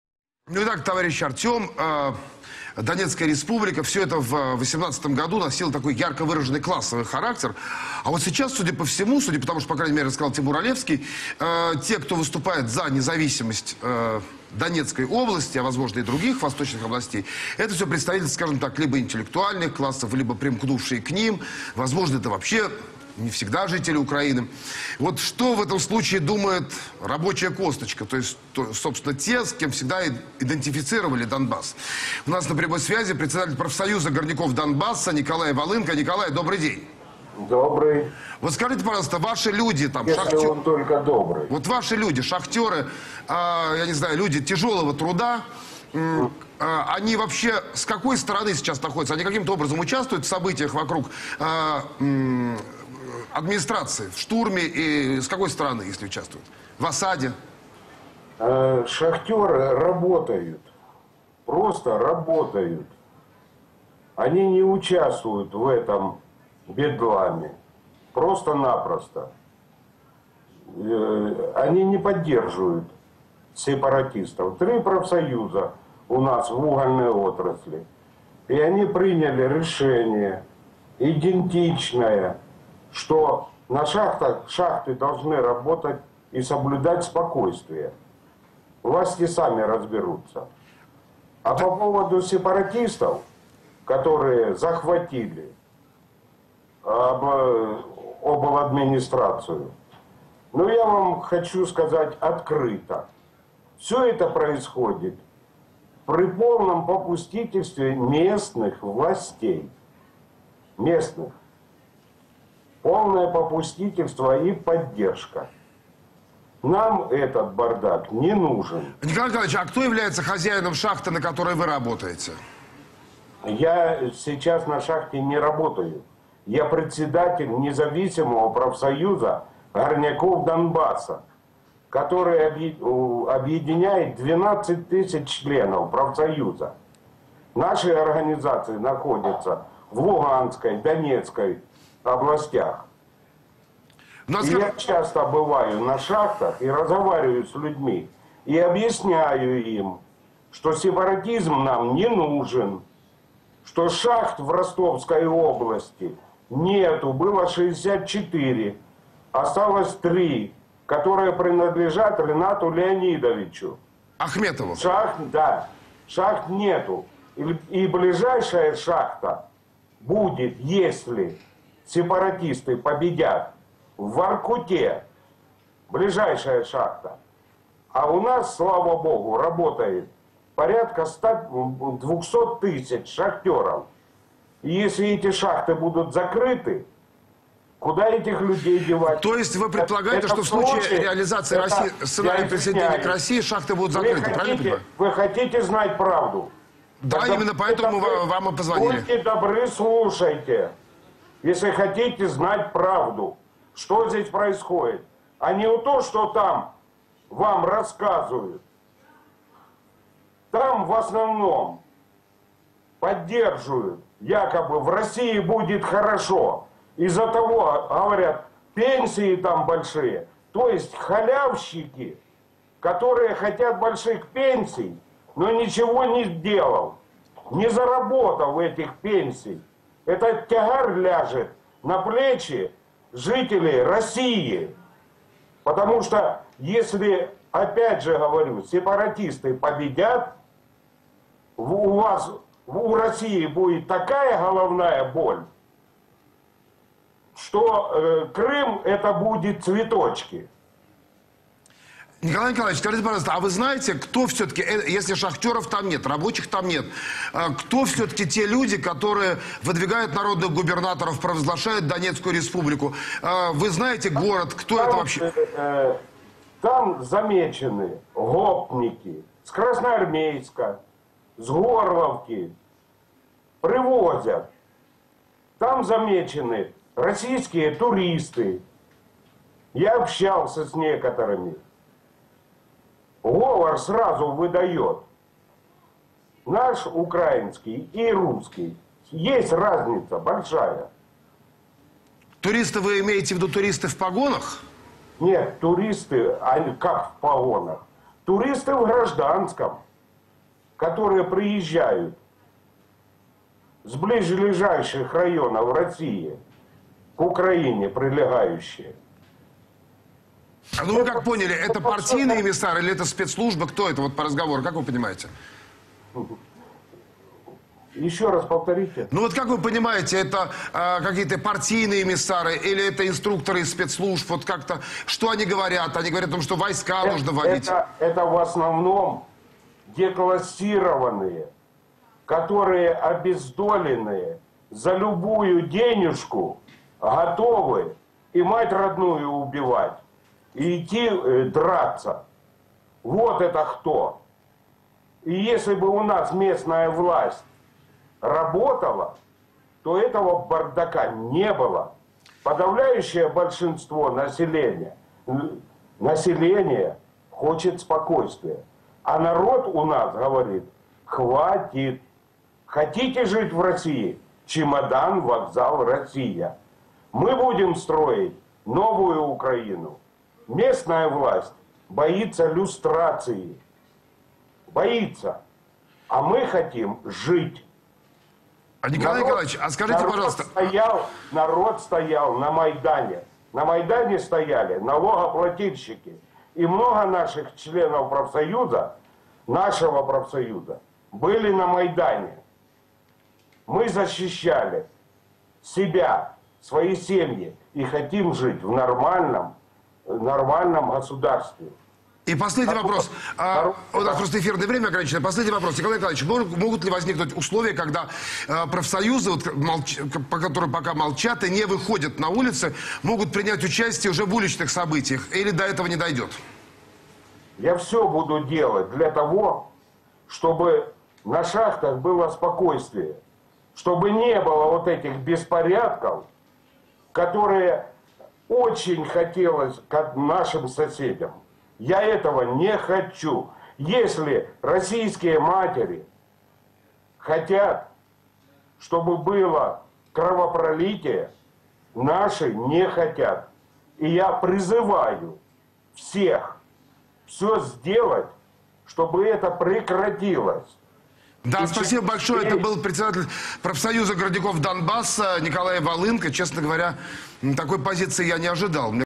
Профсоюз горняков Донбасса: «Шахтеры продолжают работать, администрацию захватили гопники»
Павел Лобков в своем авторском шоу поговорил с председателем профсоюза горняков Донбасса о происходящем у них.